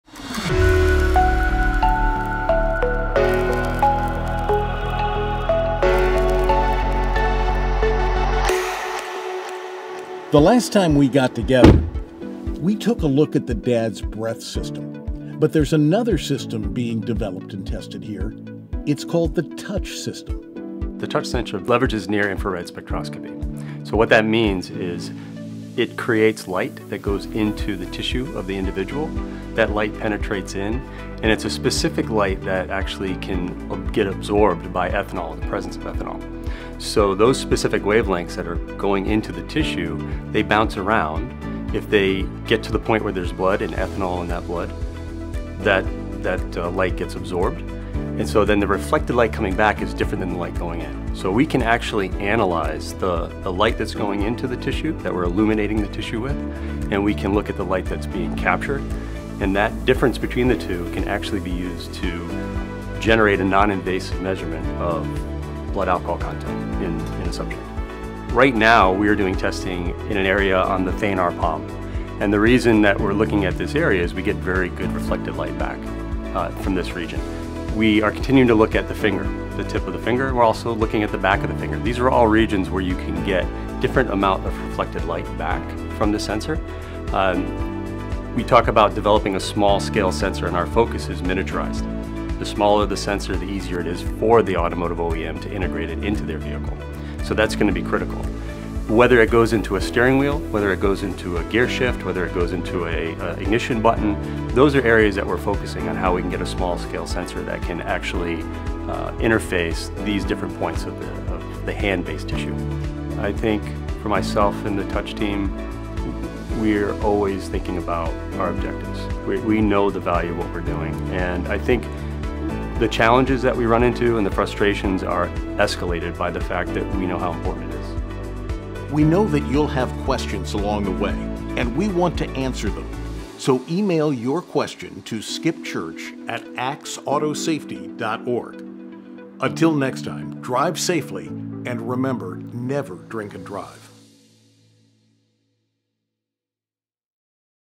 Episode 5: Behind the Scenes with Skip Church: The DADSS Touch System
Episode 5 of our video series "Behind the Scenes with Skip Church" takes a closer look at the touch technology we’re developing through the Driver Alcohol Detection System for Safety (DADSS) Program. Join host Skip Church as he goes behind the scenes to learn more about the new alcohol detection technology we are developing for vehicles of the future – what it is, how it works, how it’s been tested and how it will be deployed.